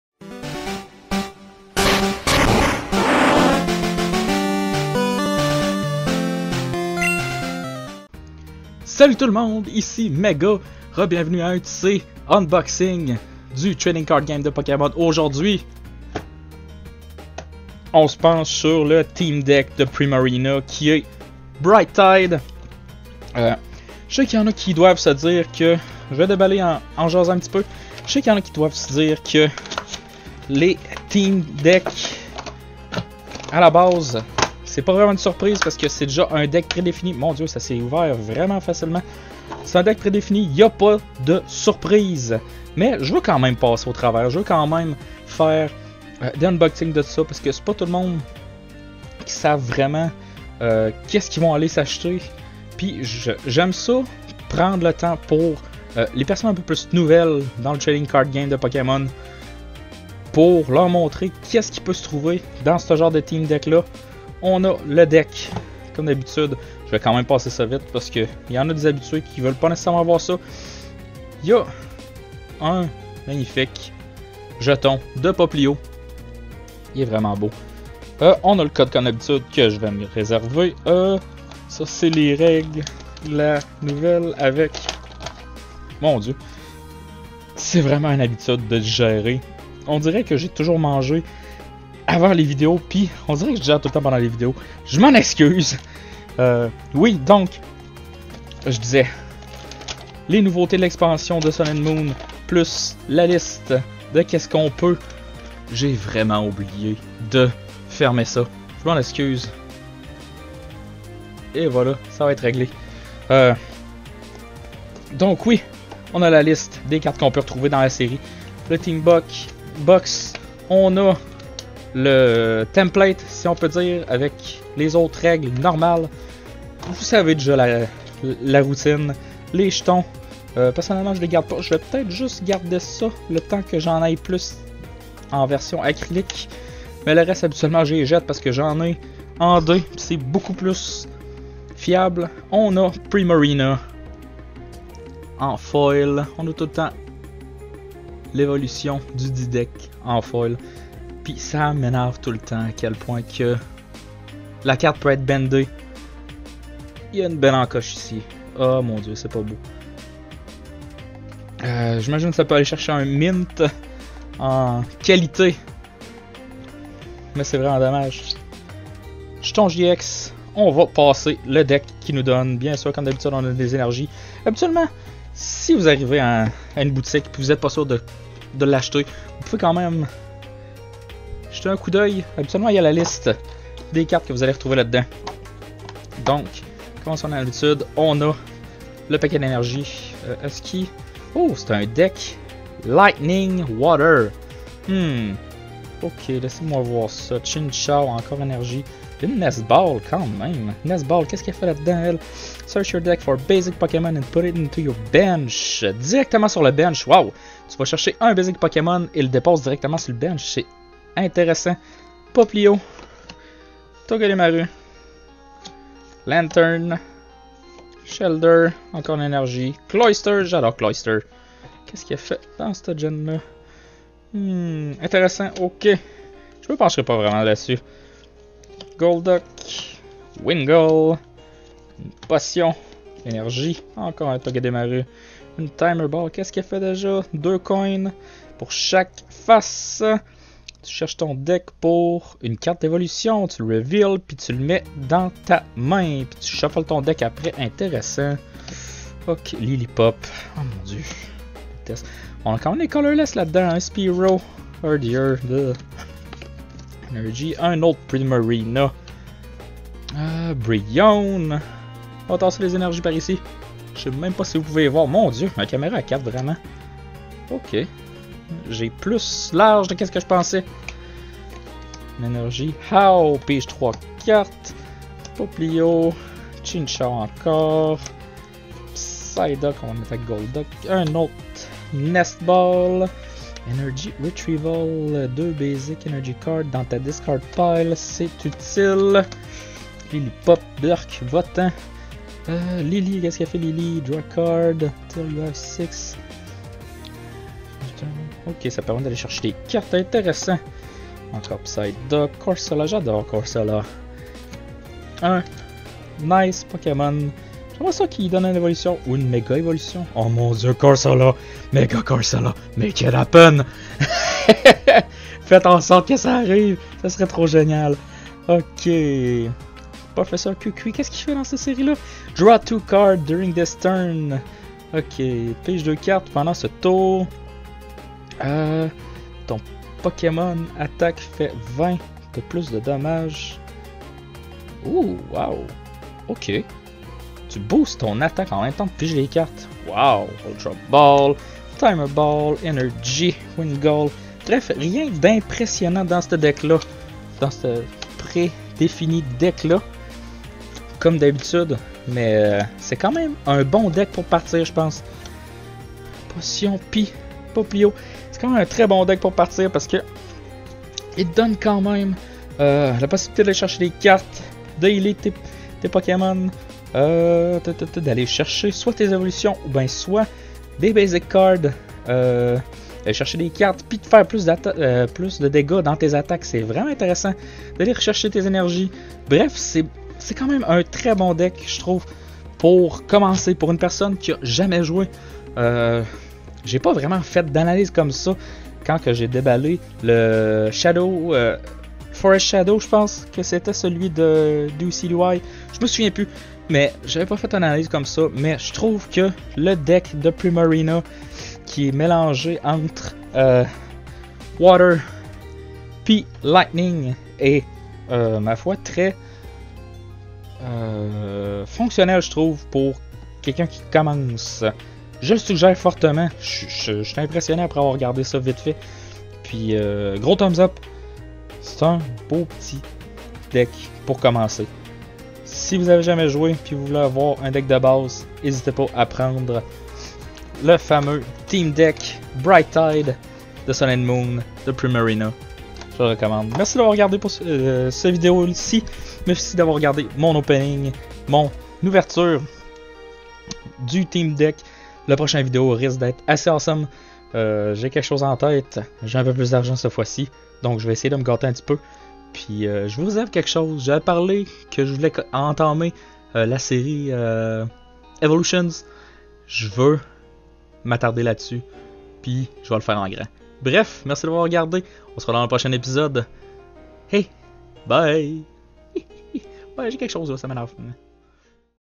Ouverture du Theme Deck Bright Tide [Pokemon TCG Sun and Moon Français Québécois #6]
Et un autre Theme Deck à mon actif! Cette fois ci, c'est celui de Bright Tide avec en vedette Primarina! Un deck préfait de type Water/Electric!

Vous pouvez me rejoindre sur...